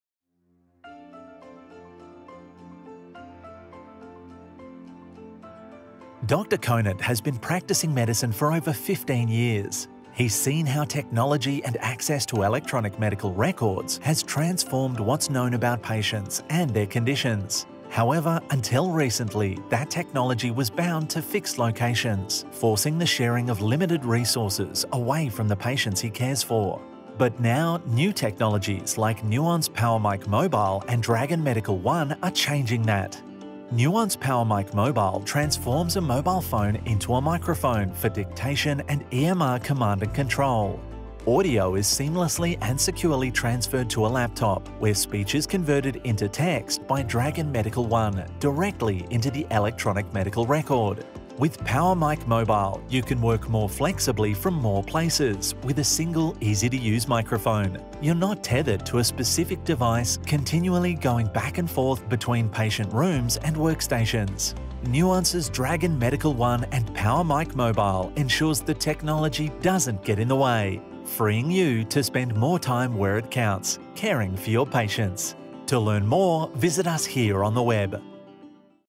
Nuance PowerMic Mobile (Australia)
Turn any workstation into a dictation station with PowerMic Mobile from Nuance - used with Dragon Medical One